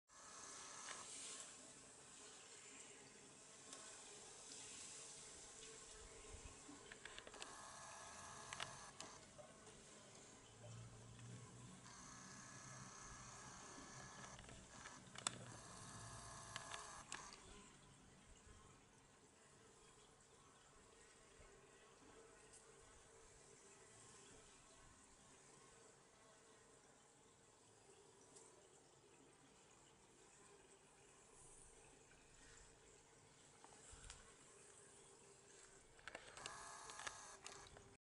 La grenouille du bassin
J ai découvert une grenouille dans le bassin .